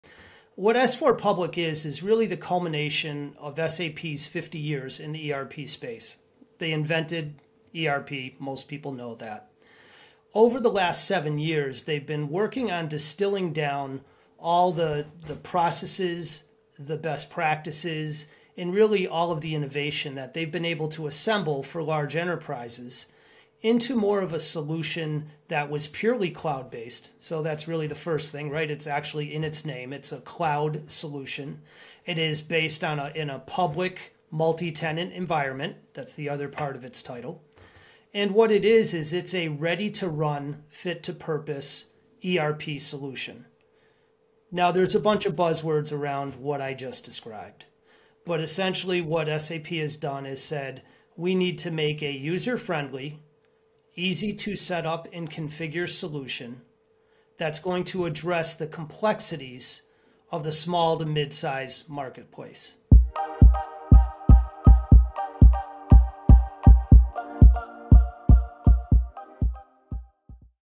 What is SAP S4HANA Cloud Public Edition, Mini Video Series Preview
Know Before You Grow Mini Video Series, Episode 3: Understanding SAP S/4HANA Cloud, Public Edition

Here is a quick snapshot of episode 3, see the video series gallery